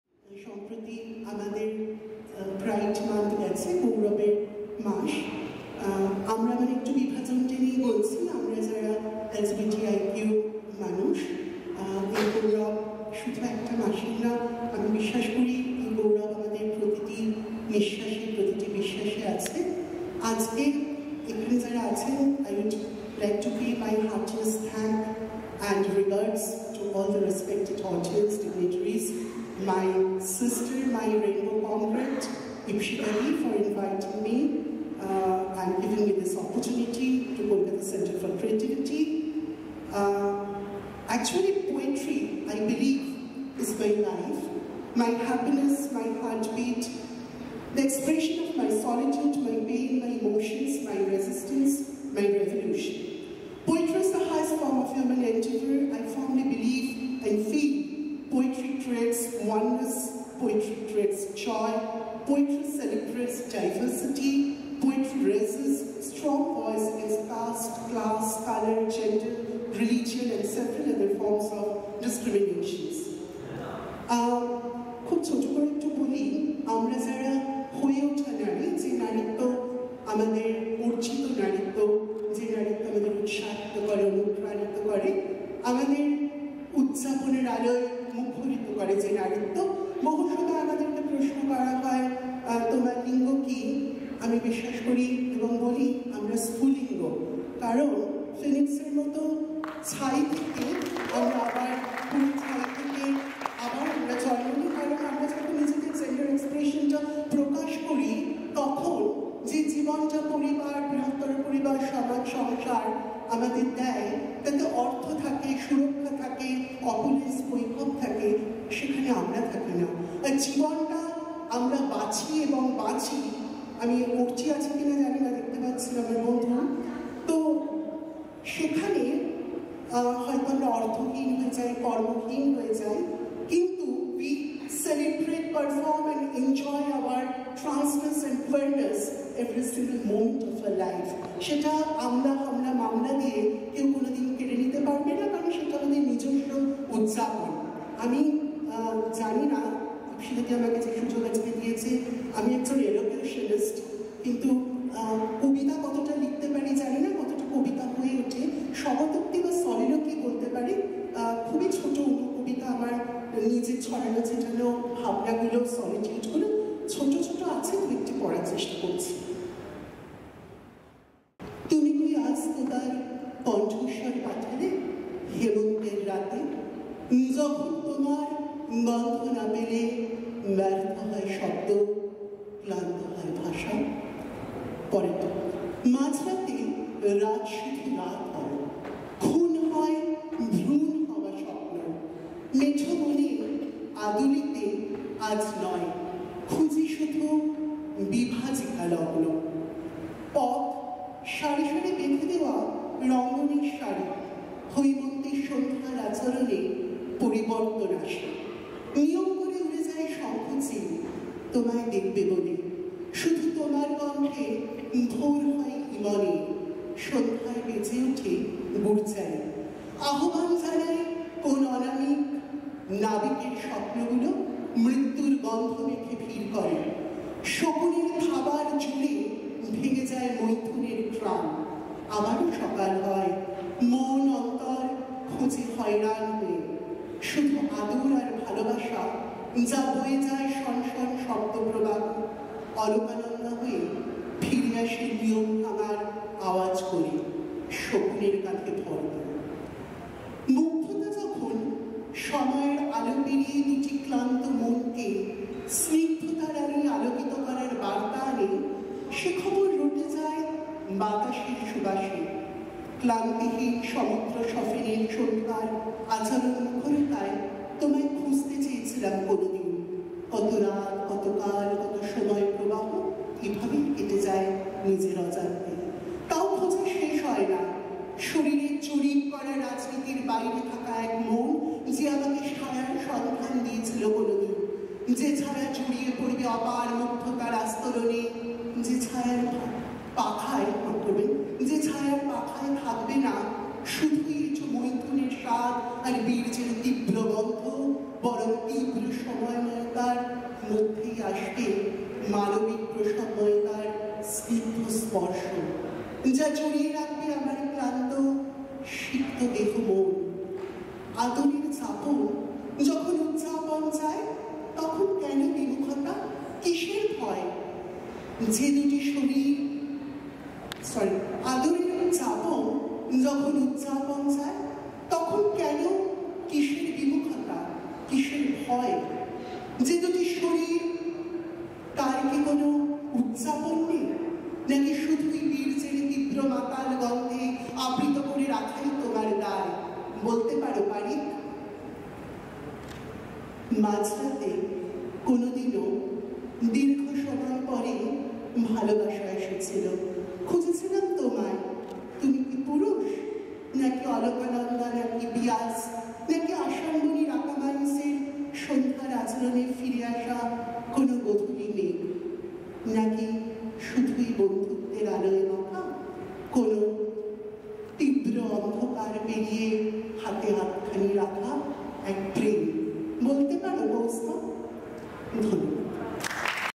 Anurag Maitrayee performing at KCC.
Anurag Maitrayee is a prominent face of civil rights and gender rights based movement, a person who is deeply inclined to believing in cultural revolution. She is also an elocutionist, recitalist, a transgender face in advertisement film making and last but not the least, an ardent reader of poems. She is associated with all kinds of movements spread across the entire country and have been working as a co-researcher within international academic platform, on gender/sexuality issues, with different people's organizations for more than a decade.